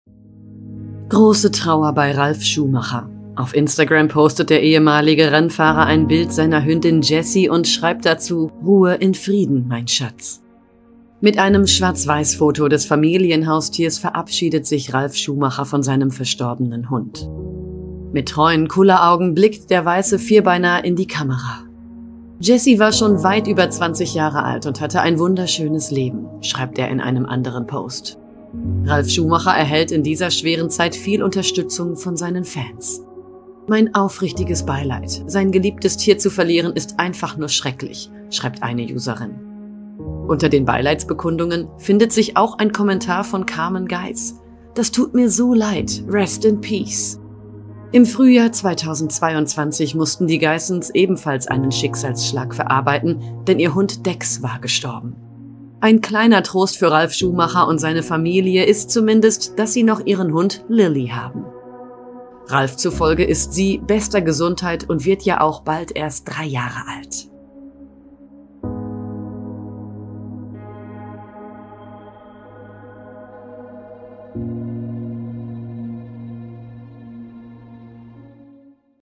Ralf Schumacher in Trauer: „Ruhe in Frieden, mein Schatz“ | PROMIPOOL
Große Trauer bei Ralf Schumacher: Auf Instagram postet der ehemalige Rennfahrer ein Bild seiner Hündin Jessy und schreibt dazu: „Ruhe in Frieden, mein Schatz“.